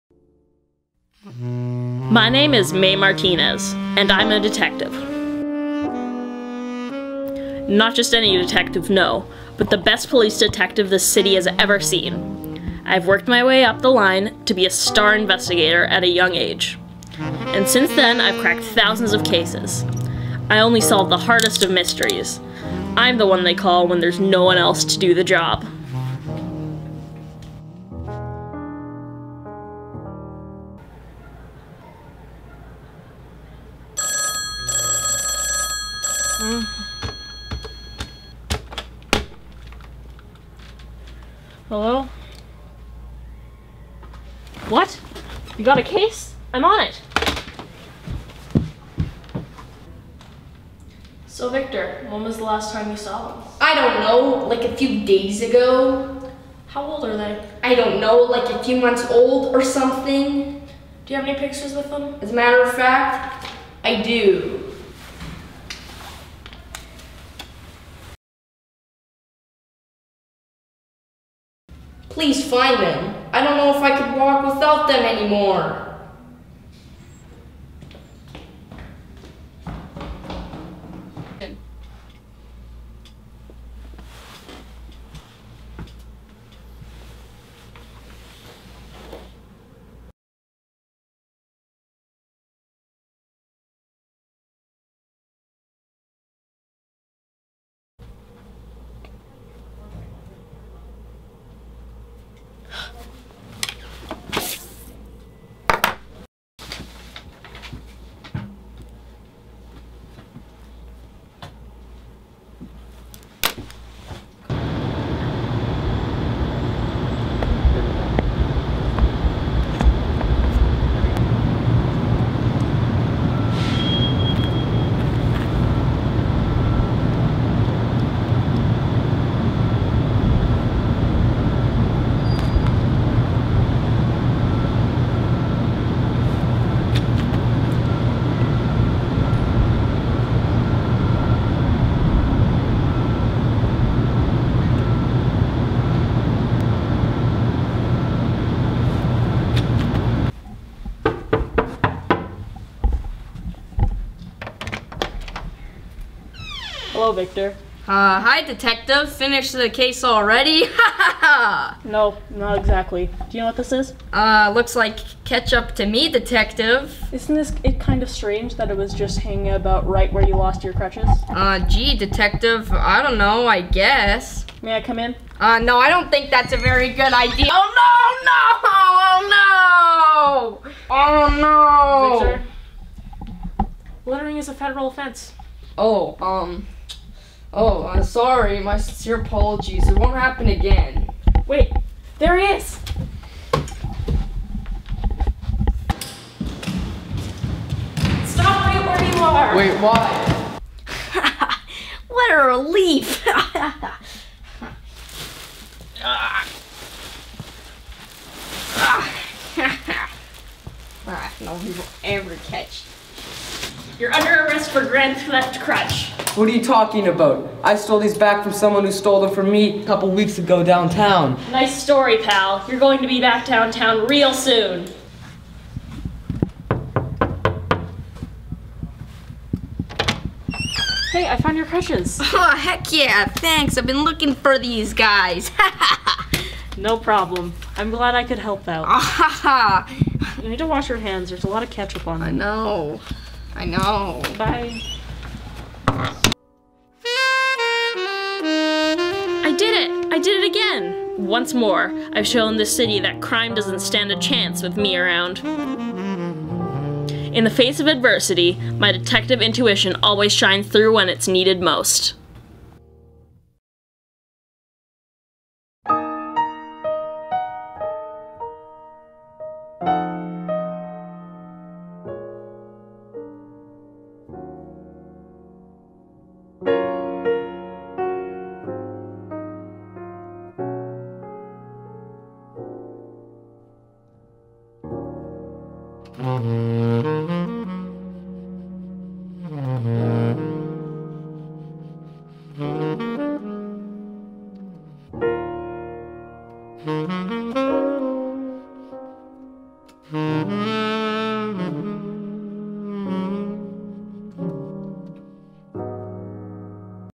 Film Camp 2018 - Grand Theft Crutch
A seasoned detective attempts to crack one of her hardest cases yet. However, there may be more to it than meets the magnifying glass.

Cast: Aida Dalton, Andrew Curley, George Woollam, Nolan Hall
Crew: Elijah Brass, William Badger, Nolan Hall, Andrew Curley, George Woollam